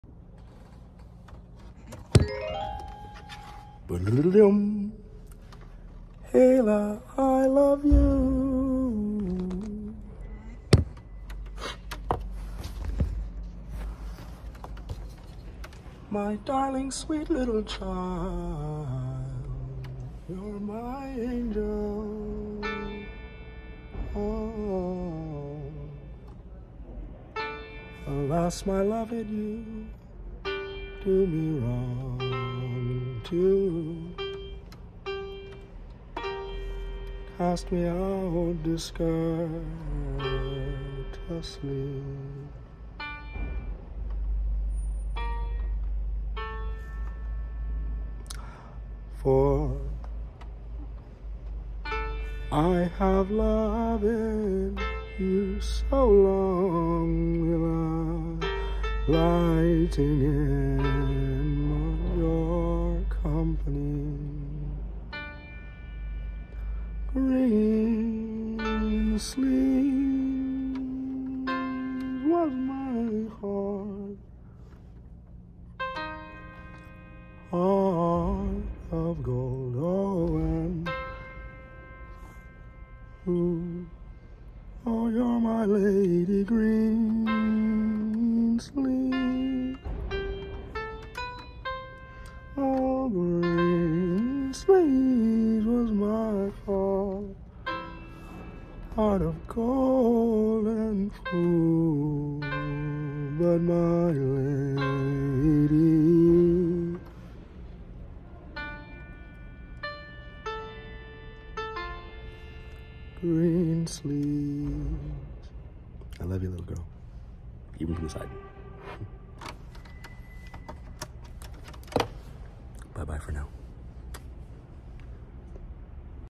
Lemons to Lemonade 4
Sometimes, God lets us go through awful trials. This series is born out of times when FaceTime is withheld. It’s an awful sad time when Ayla and I can’t connect. No matter what, I love my little girl and will pray for my family always.